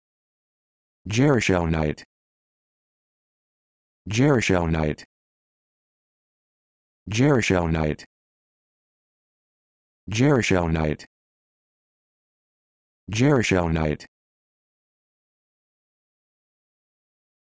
How to Pronounce Gershonite
This video shows you how to pronounce Gershonite